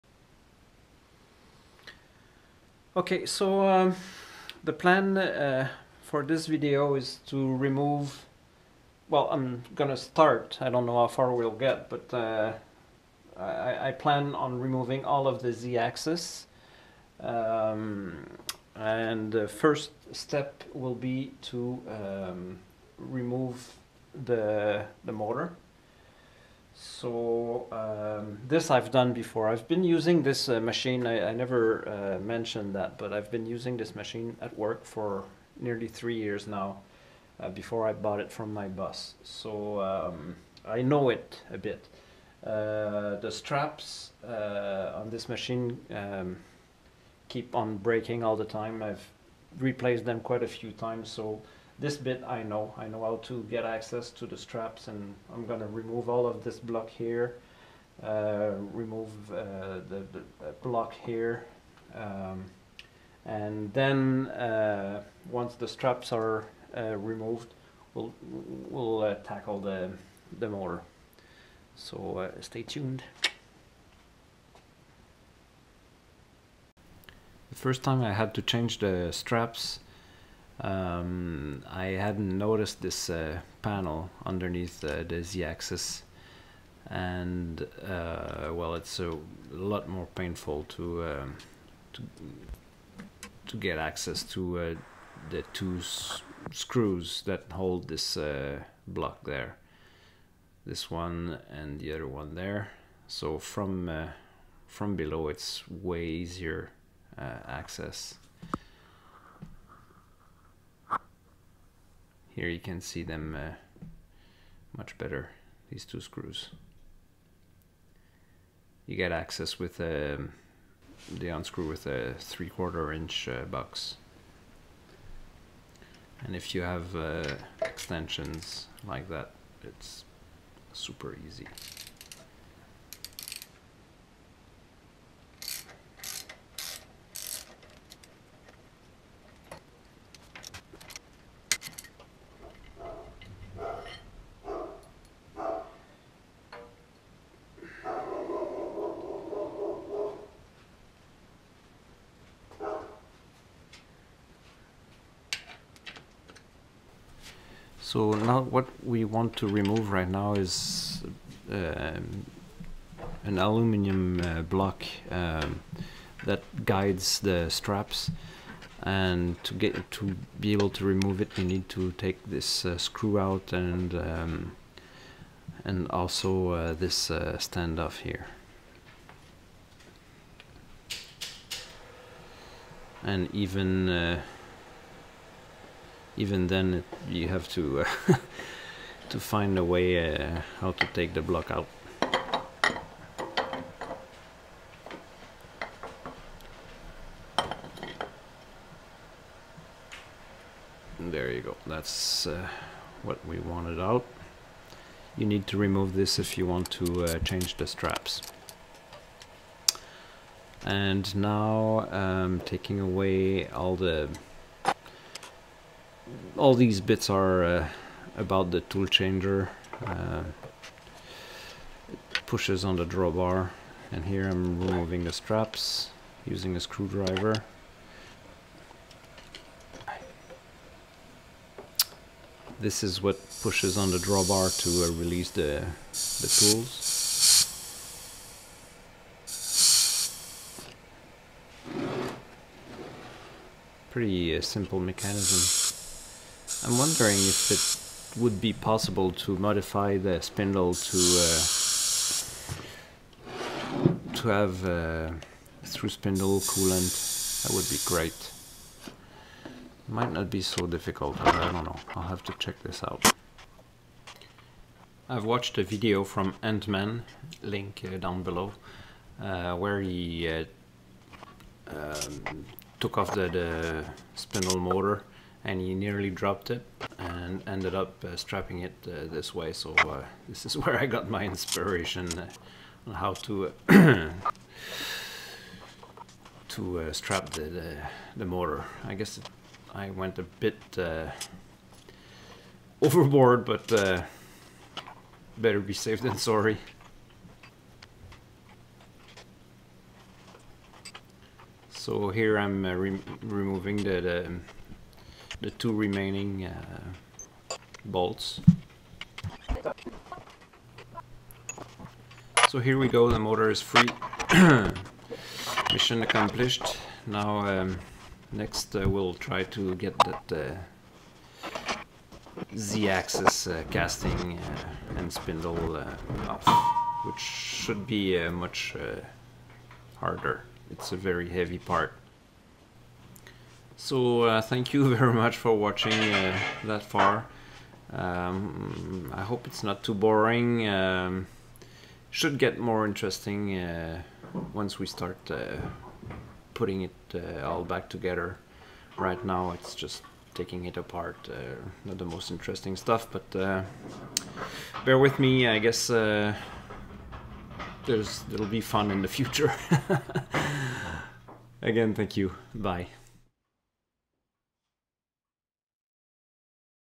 Part 3 - (Removing the spindle motor)
Part 3 of the Fadal VMC 4020 reconditioning series.
Removing the spindle motor.